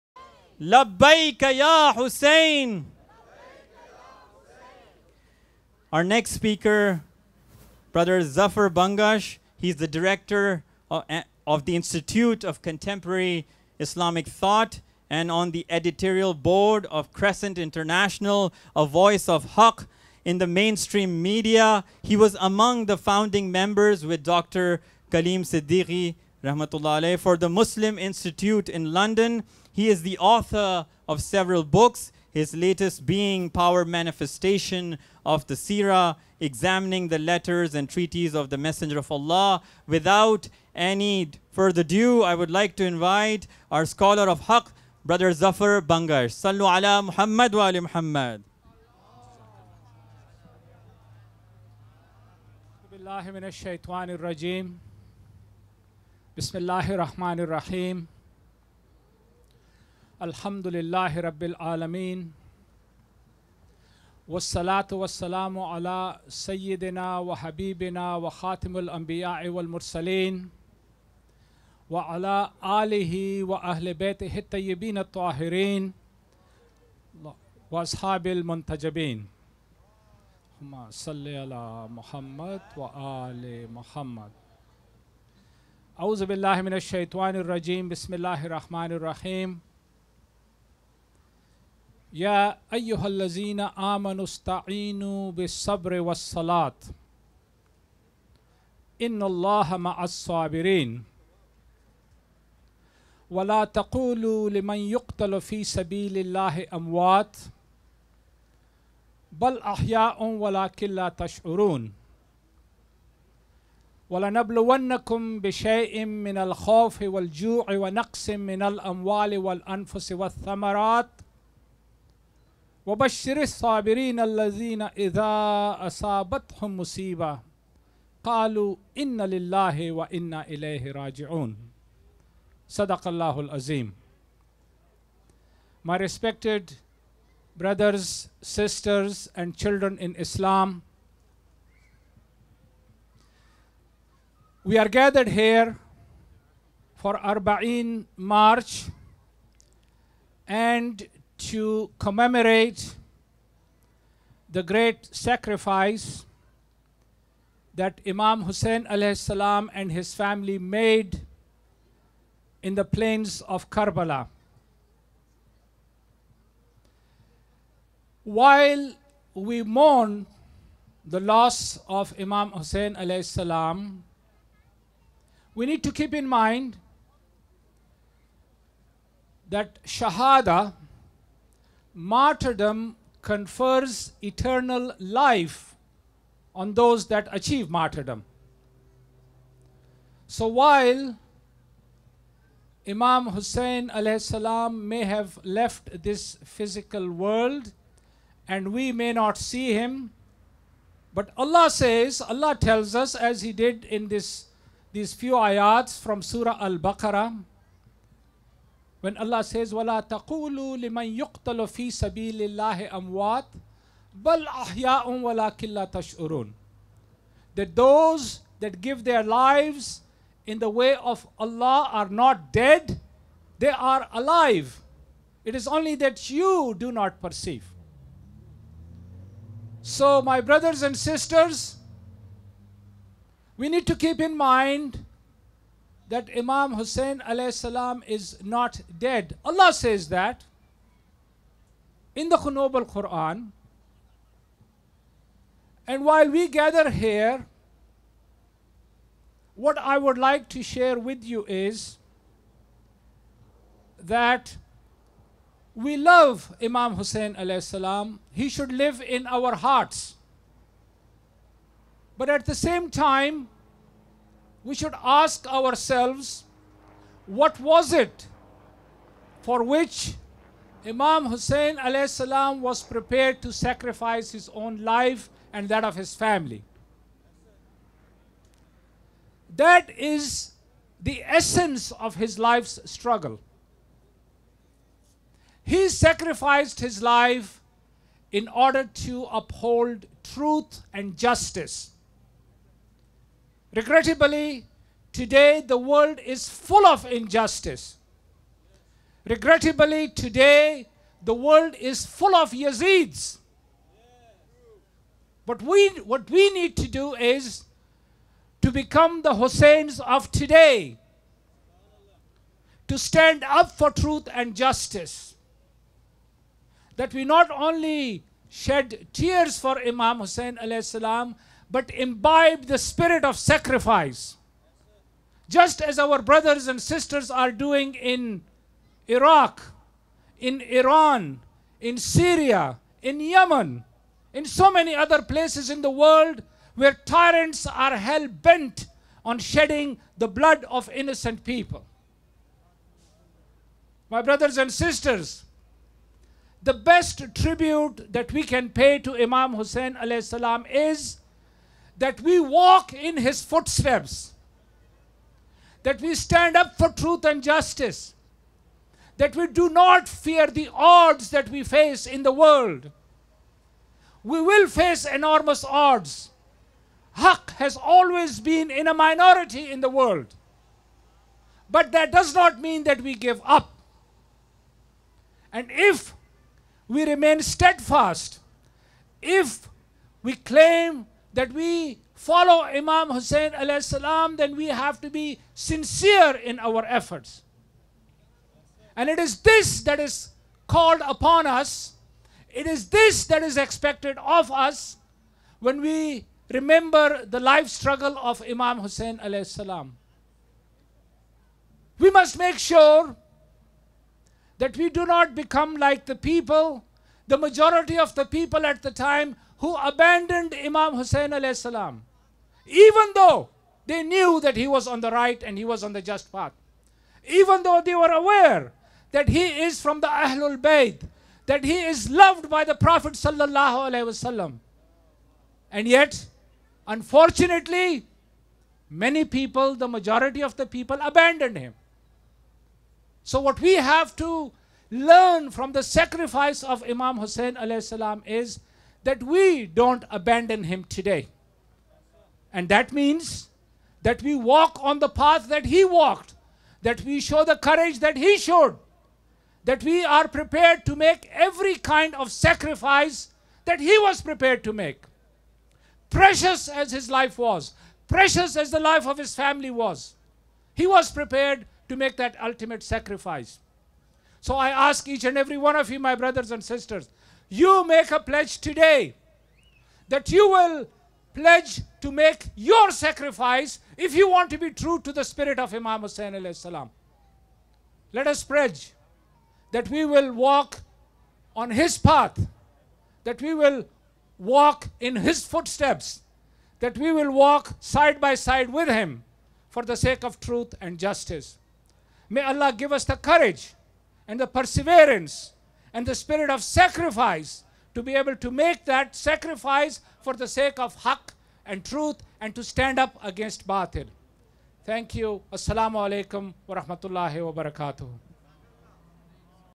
Imam Zafar Bangash - Toronto Arbaeen Walk 2021 - Canada
Toronto Arbaeen Walk - Canada

Thousands of people including children’s, elders and youths gathered at 1 Strachan Avenue to commemorate local Arbaeen walk in Toronto. Global Arbaeen walk is organized every year on the fortieth day of Ashura where 20 million people from across the globe participate and walk from Najaf to Karbala in Iraq.

Ashura is the day when Imam Husayn, (Grandson of Prophet Mohammad (saw) and a seventh century revolutionary leader) along with his seventy two companions was killed on the plain of Karbala by the tyrant Yazeed.

At the gathering point, there were speeches , spoken words and poetry's to highlight Imam Husayn’s dignified stand for social Justice and Lady Zainab’s courage and bravery in saving Islam post Ashura. The theme of this year walk was to walk on the path of Islam, manifested by Imam Hussain (a).

The participants walked from 1 Strachan Avenue to 361 University Avenue carrying placards, chanting slogans, reciting spoken words, eulogies  and short speeches along the way. They carried the message of Lady Zainab, it said “ in this sacrifice and revolution, I see nothing but the beauty of God”. The youths were distributing information leaflet pertaining to Arbaeen to the passerby. At the end, all participants send their salutations to the martyrs of Karbala and vowed to remember their sacrifice and walk in their footsteps.